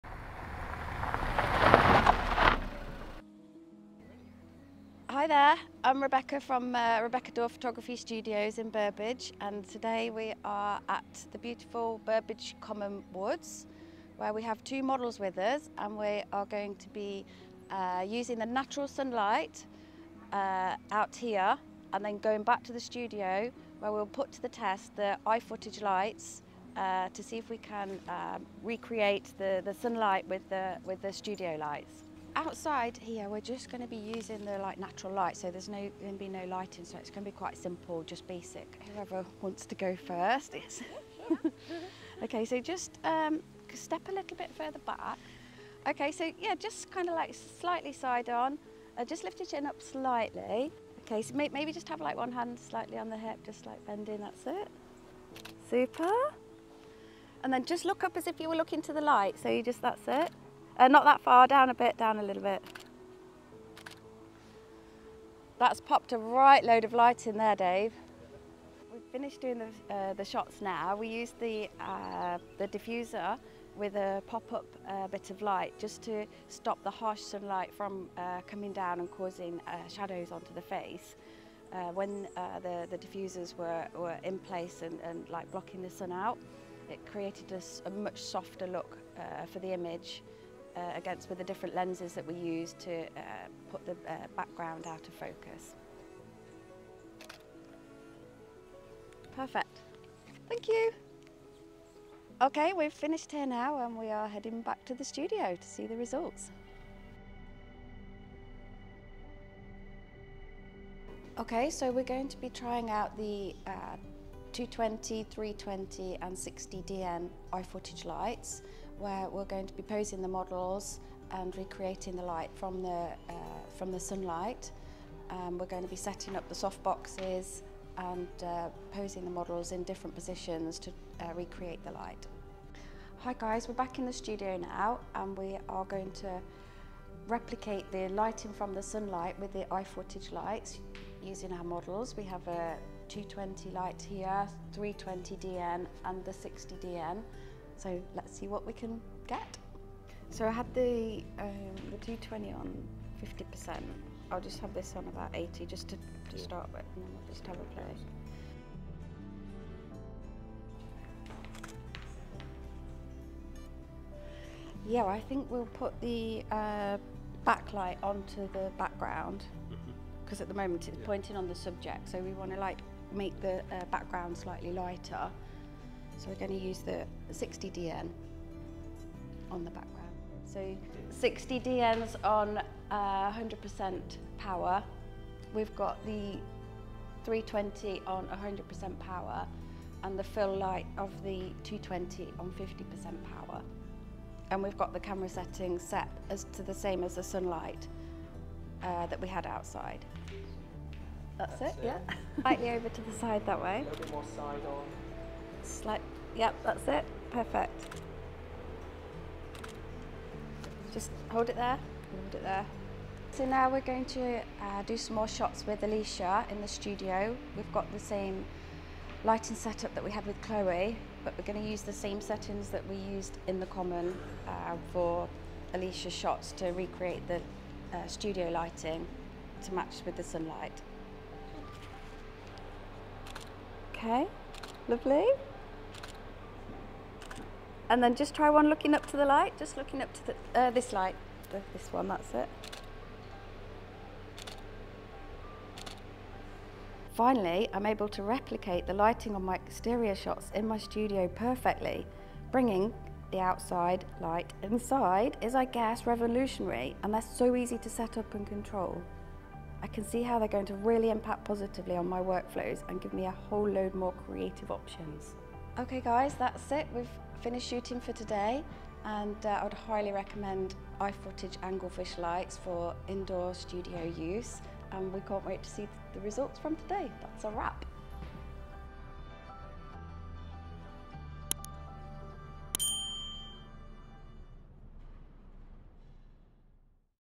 Explore the possibilities of Daylight Natural LED lighting with Rebecca Dawe | iFootage Lighting
Tune in and immerse yourself in the captivating world of photography with the incredibly talented Rebecca Dawe. Witness firsthand the astonishing potential of dependable daylight natural lighting and discover the breathtaking possibilities it unveils.